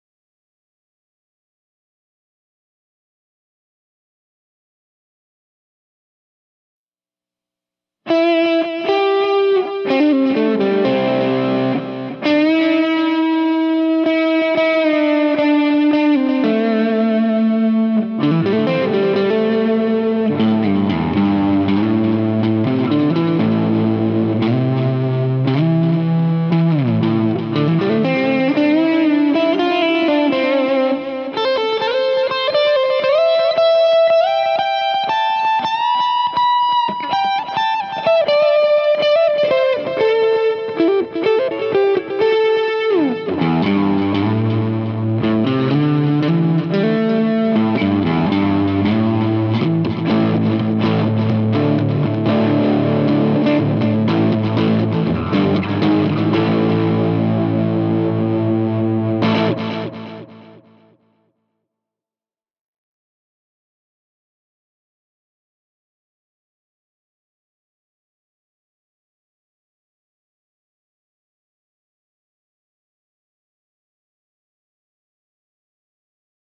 Heather Brown Electronicals The Blessed Mother Unboxing
Let's take a look at the wonderful Blessed Mother Boost Overdrive from Heather Brown Electronicals, shall we? This tone-enhancer has a secret weapon up its sleeve, setting it apart from your normal drive pedals - let's see what's inside...